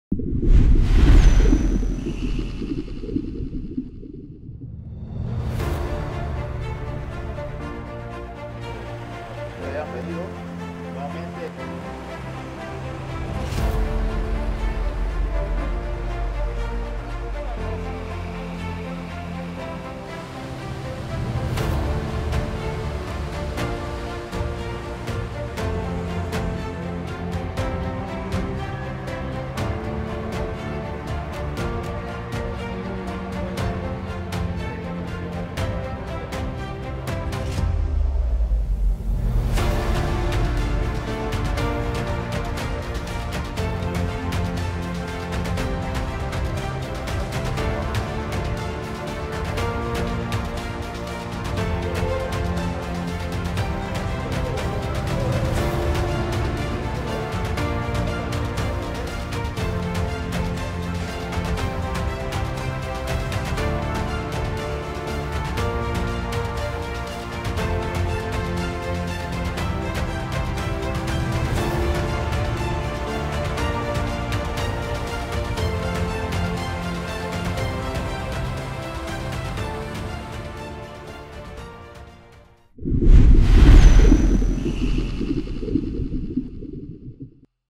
Director Ejecutivo del IDAAN visita la planta de Mendoza.
Director Ejecutivo del IDAAN visita la planta de Mendoza, bajo la administración de @canaldepanama en La Chorrera como parte de un recorrido por las instalaciones de producción hídrica en el distrito.